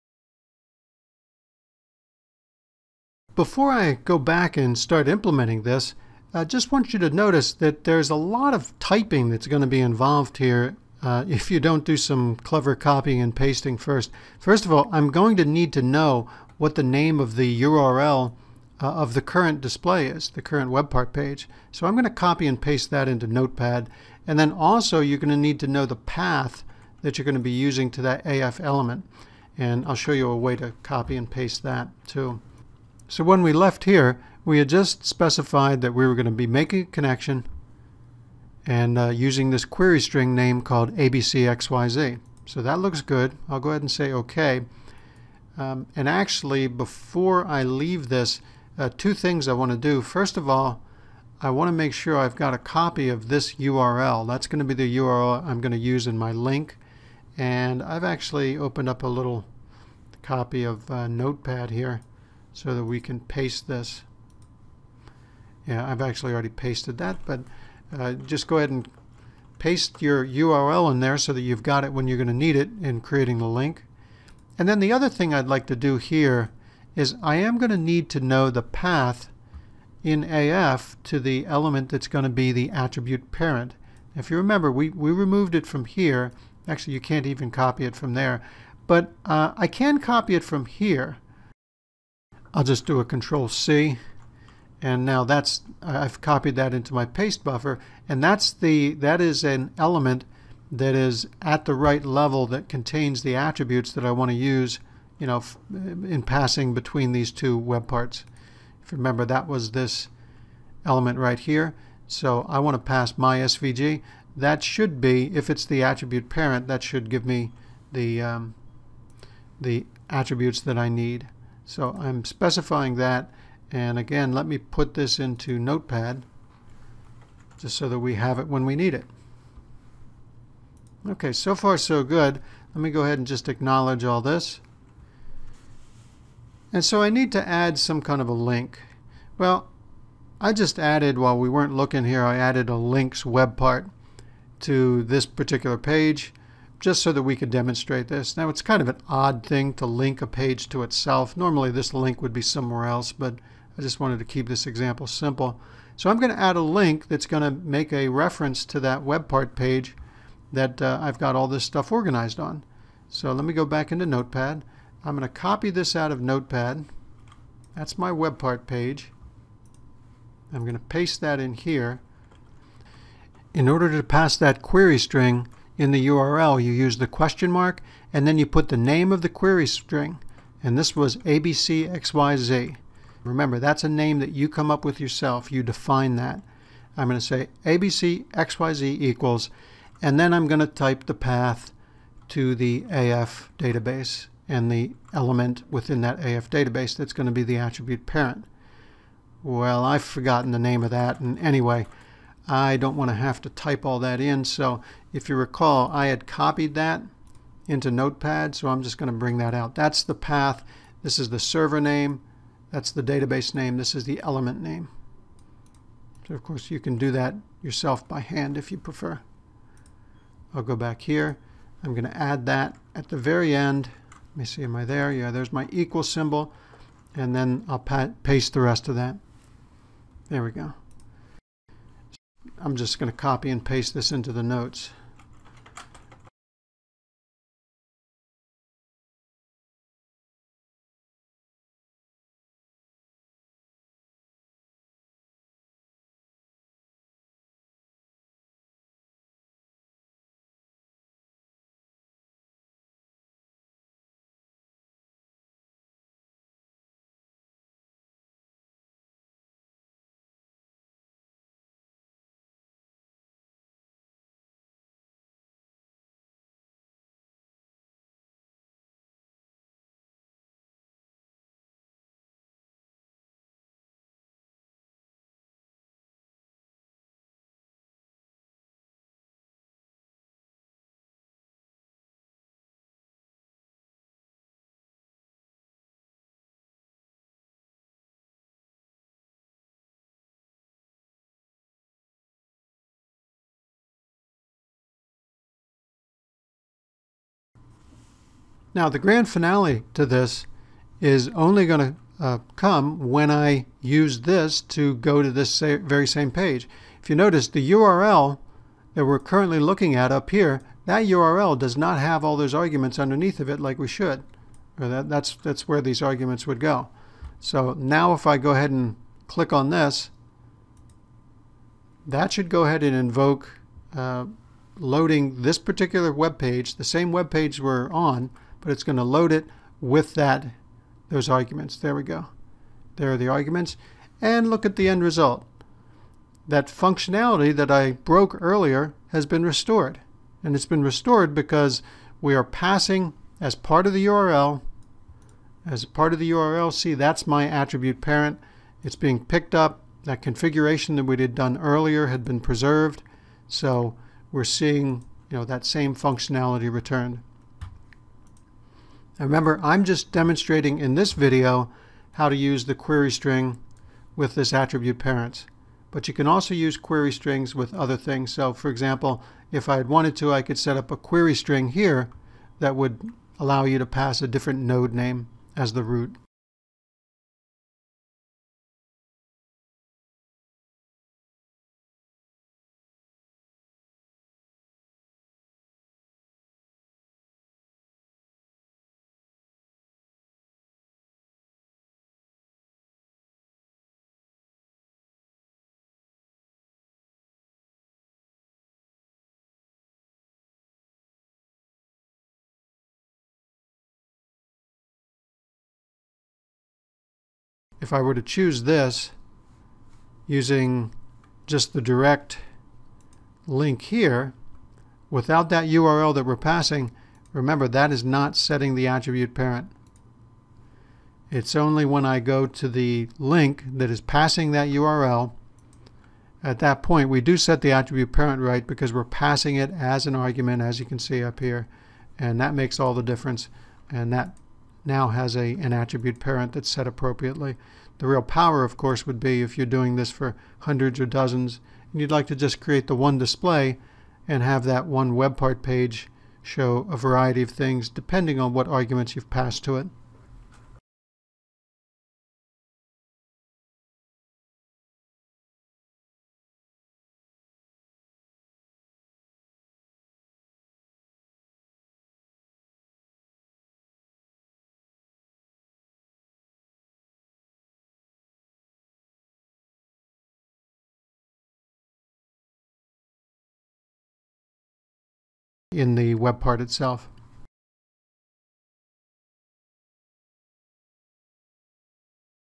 OSIsoft: Use Querystring Parameters with the PI TreeView Web Part, Part 2. v2010
Querystring parameters can be added to a web part page URL to send parameter connection values to consuming web part fields when the page first loads.

you can see where this video is in the sequence of videos on this topic.